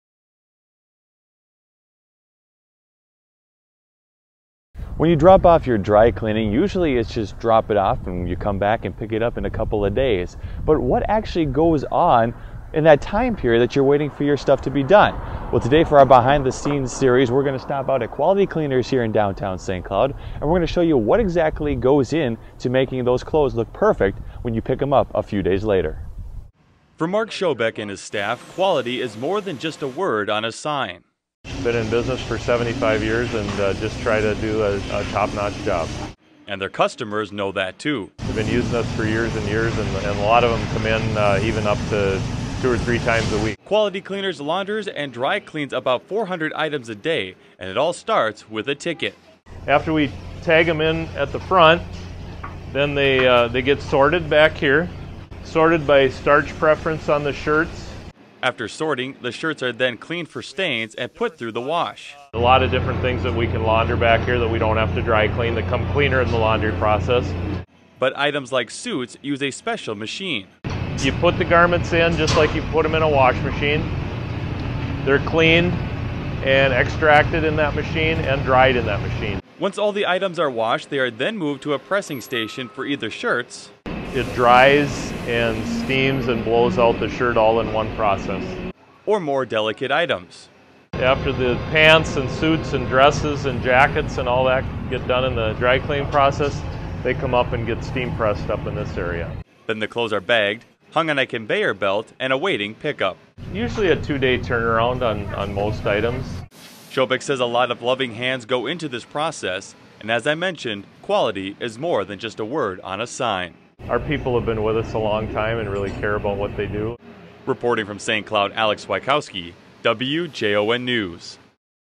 Behind the Scenes: Putting Quality Care in Your Suits And Laundry [VIDEO]
This week in our "Behind the Scenes" series on WJON, take dry cleaning to another level at Quality Cleaners.